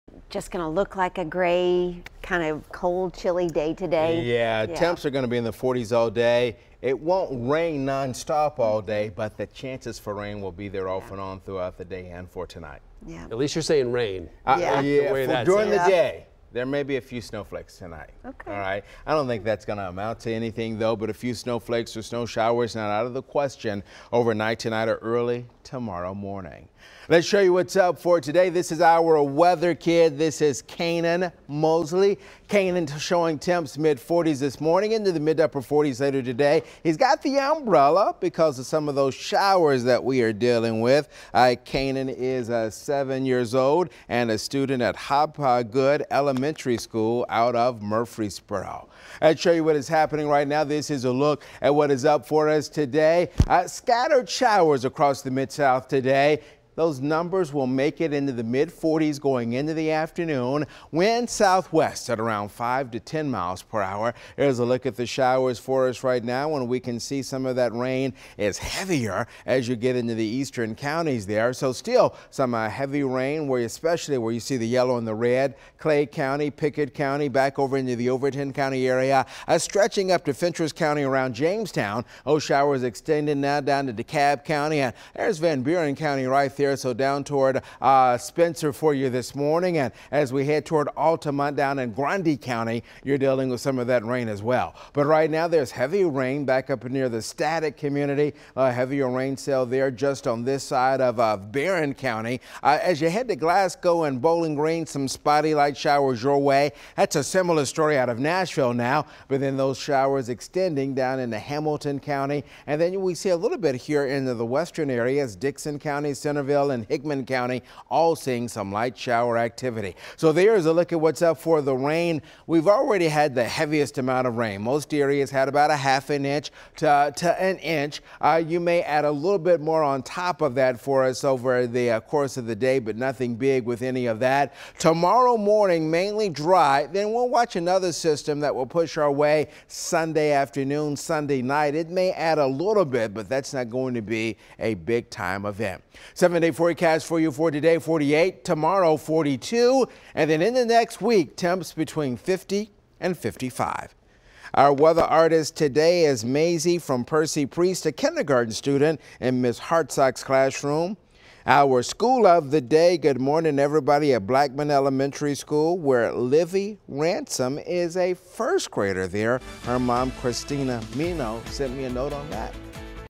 Weather Kids: Friday, January 24, 2020
Cainan Moseley is today's weather kid for Friday, January 24, 2020.